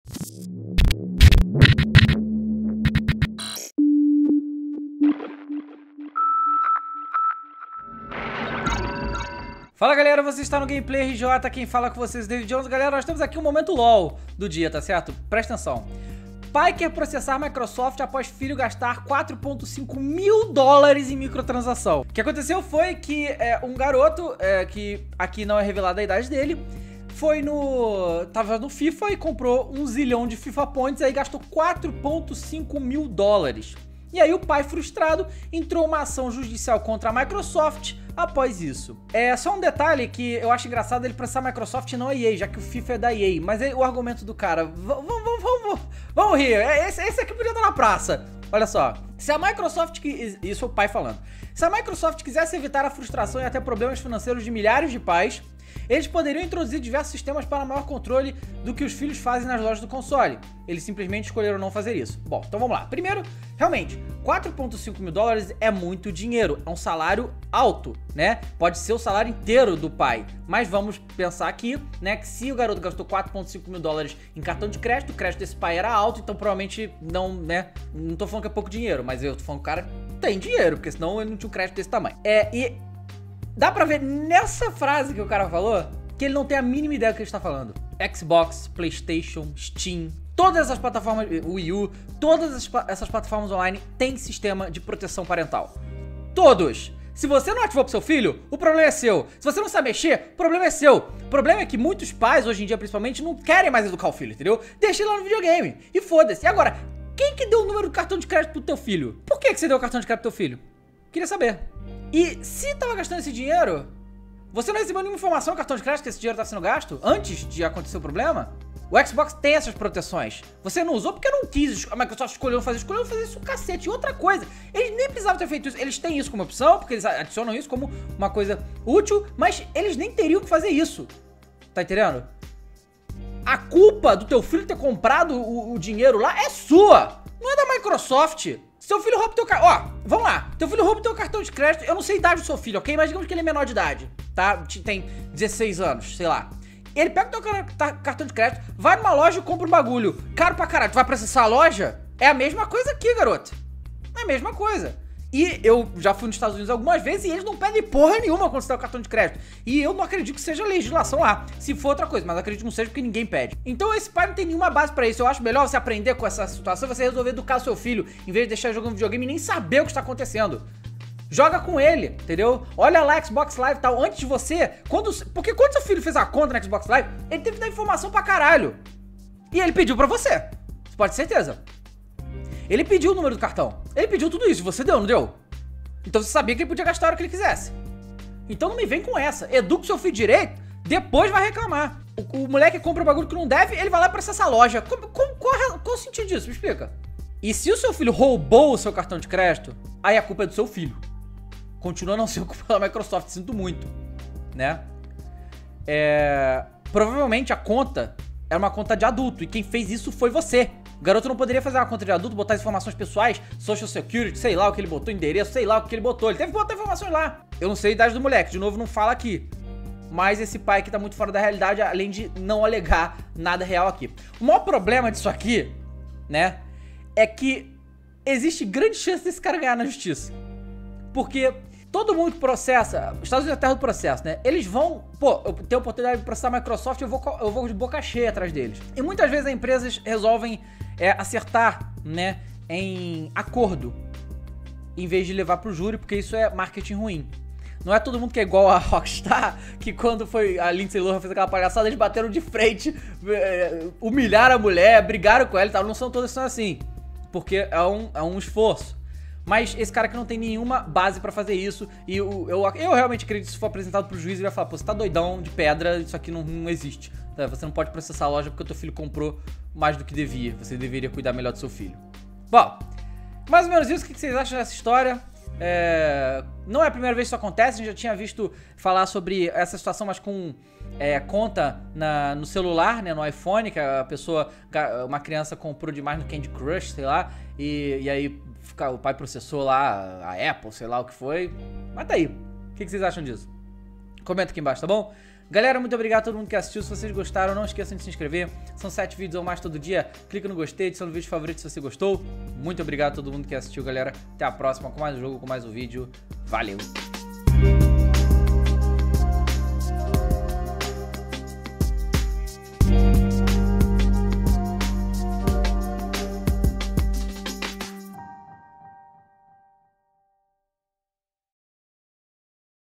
Filho gasta mais de R$ 14.000 no FIFA e pai processa MICROSOFT, LOL
Os Piratas